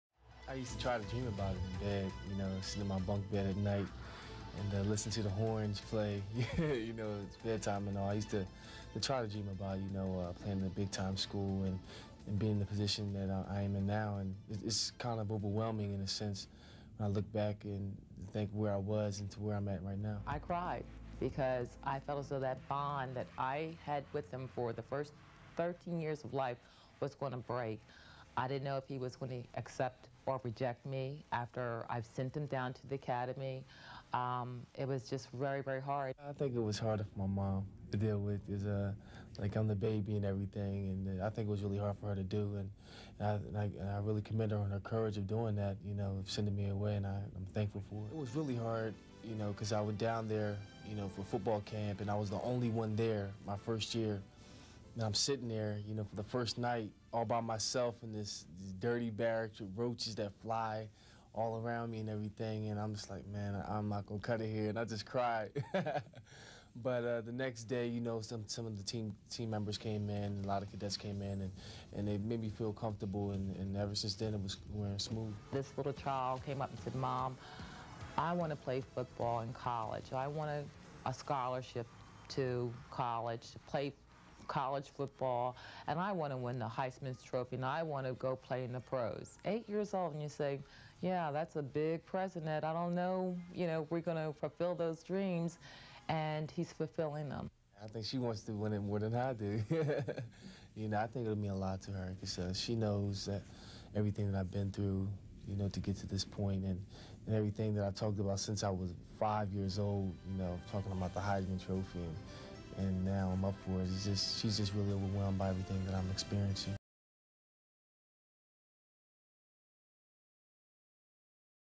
Eddie George and his mother sit down and talk about the Quarterback's past
Eddie George and his mother talk about his past and how it shaped him to in a Heisman Trophy winner in 1995.